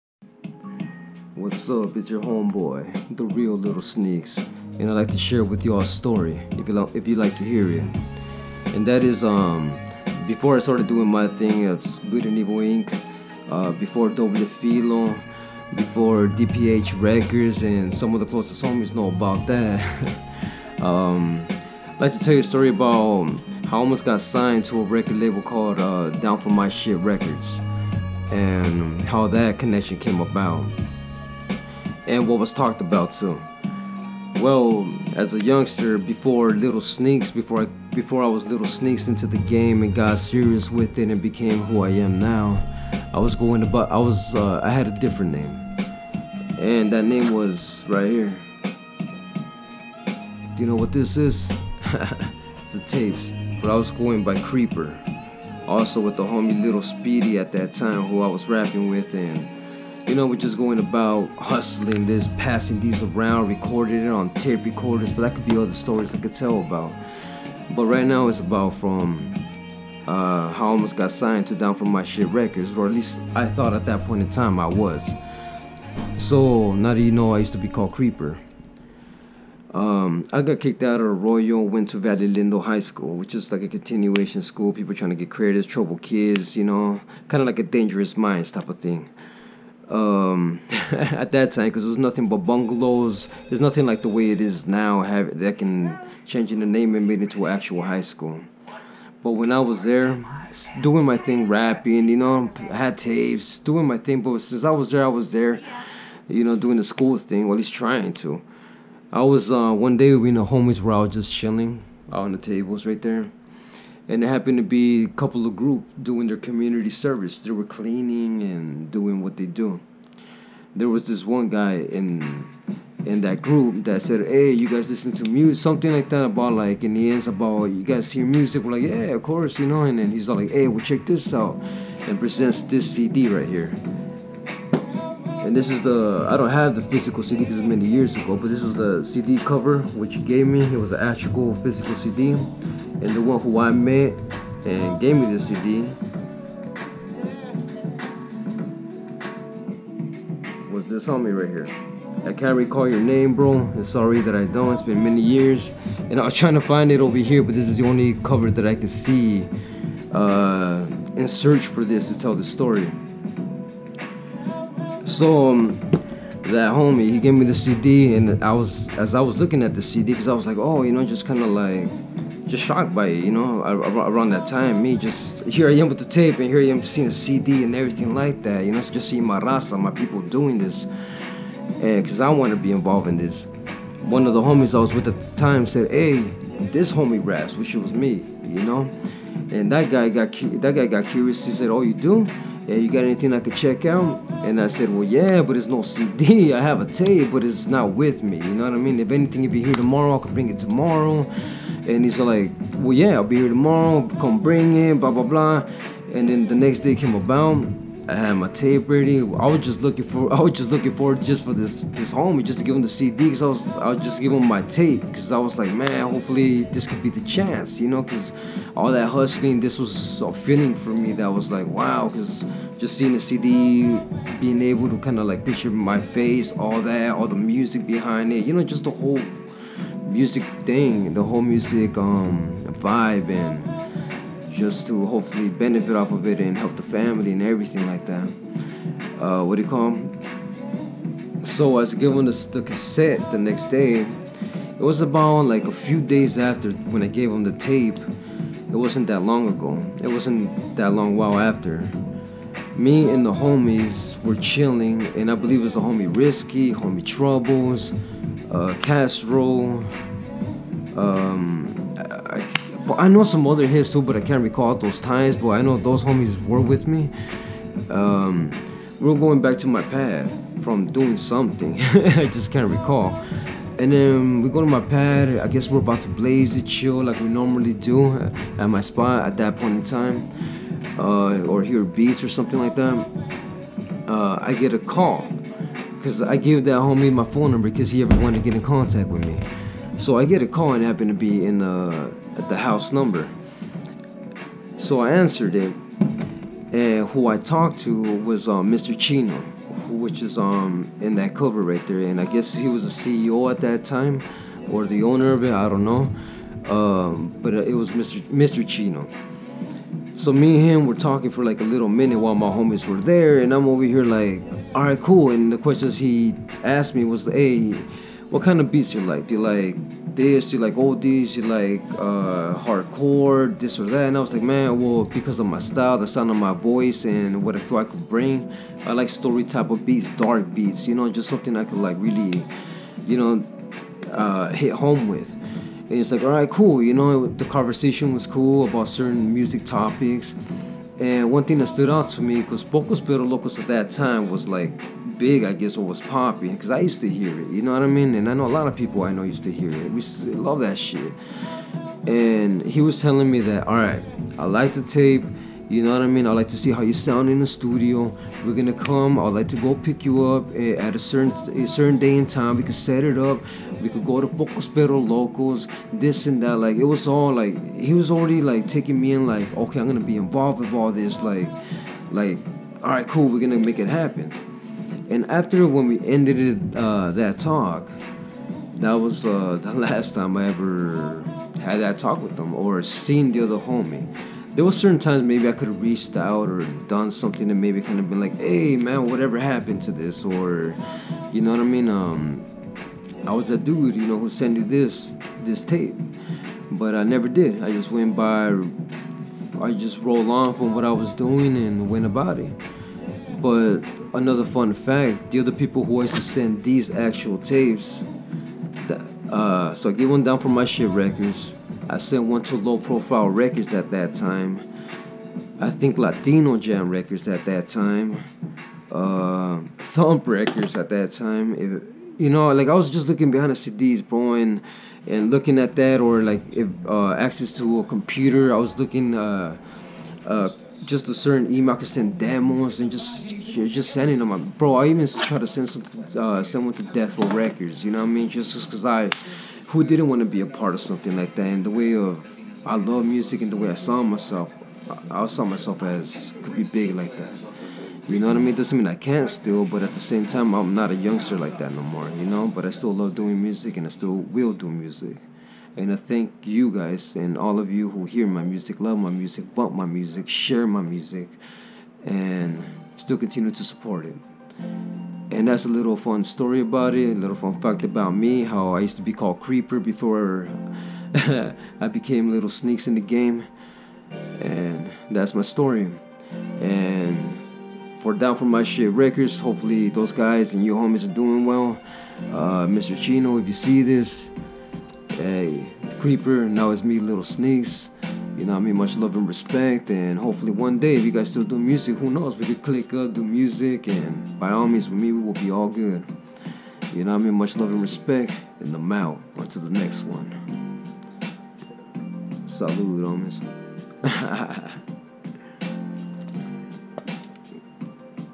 Story Time #1 - Story about almost getting signed to Down For My Shit Records in his teen years
Here is the artist the real  lil sneaks talking about a time when he was young and almost getting singed to a label called down for my shit records. Sneaks also talks about what use to be his rapping name and shows his very first tape with songs in which he had recorded in his younger years along with another rapper named lil speedy.

Its about time i let some story's out and let the world and music fans know about me and the struggles that i have faced or situations i was in. Had to use my other cell with the cracked screen lol  to show the picture of the cd as i was using my unbroken cell to record this.
If you have not check out Lil Sneaks and his music feel free do do so